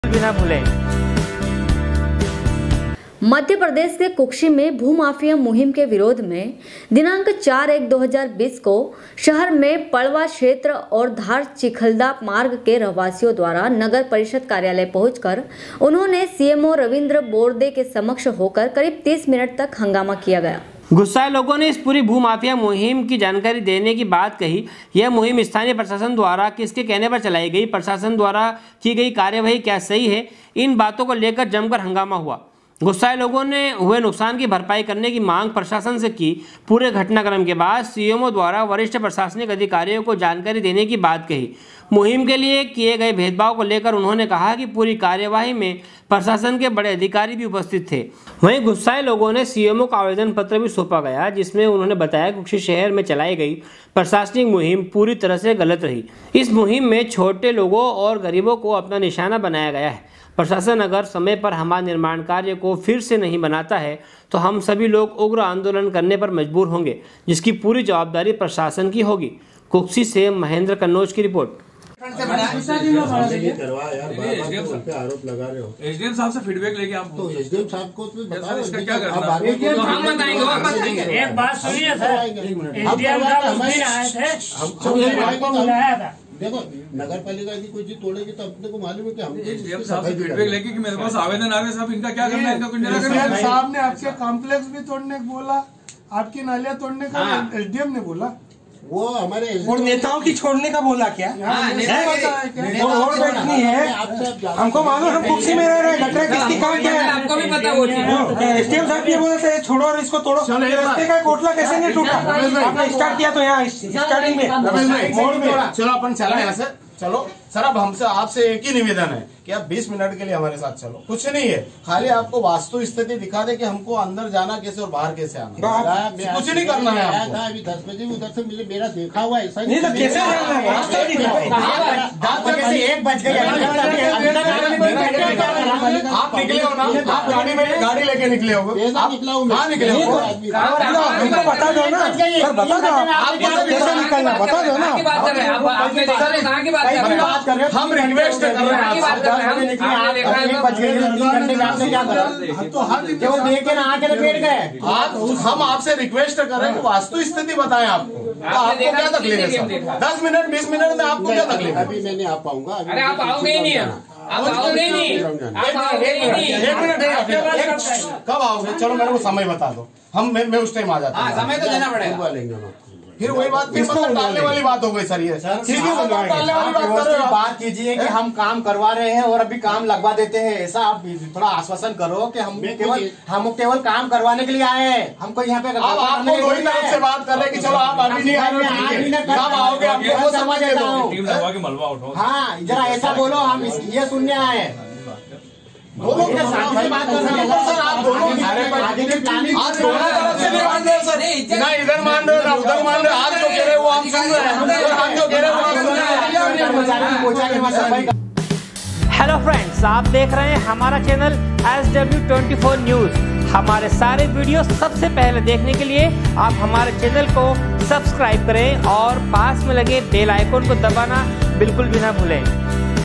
sw24news kukshi, 05.01.2020
मध्यप्रदेष के कुक्षी में भू माफिया मुहिम के विरोध में दिनांक 4.1.2020 को शहर के पड़वा क्षेत्र और धार चीखल्दा मार्ग के रहवासियों द्वारा नगर परिषद कार्यालय पहुंचकर उन्होंने सीएमओ रविंद्र बोरदे के समक्ष होकर करीब 30 मिनट तक हंगामा किया गया कुक्षी से महेन्द्र कन्नोज की रिपोर्ट#sw24news